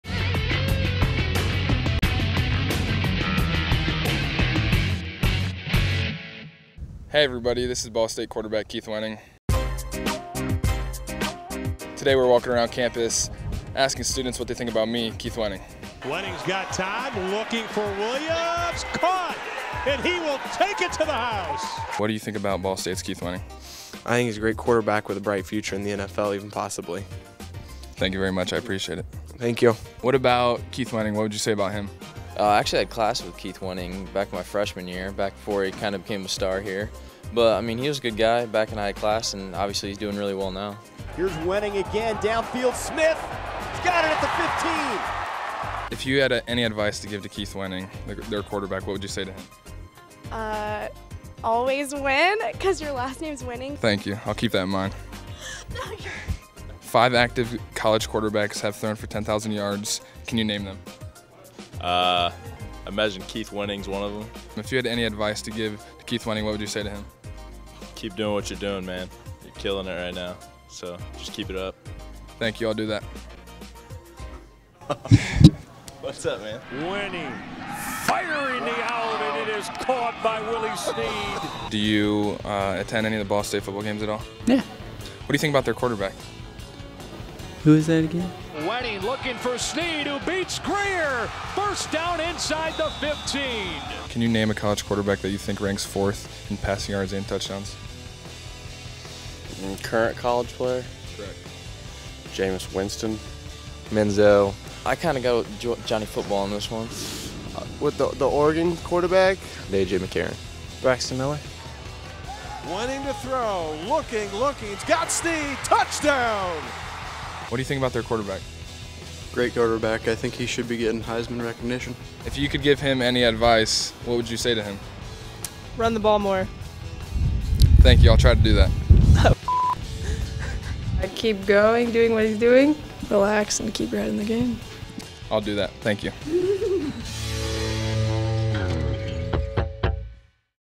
Keith Wenning: The Best Quarterback You May Not Know
Ball State's Keith Wenning leads the nation in 300-yard passing games.  He is second in the nation in total passing yards.  And yet, you might not recognize his name in a sea filled with the likes of Johnny Football and Jameis Winston.  Find out more about one of the most "hidden gem QBs" in college football...and if people would even recognize it if they were talking to him on the street.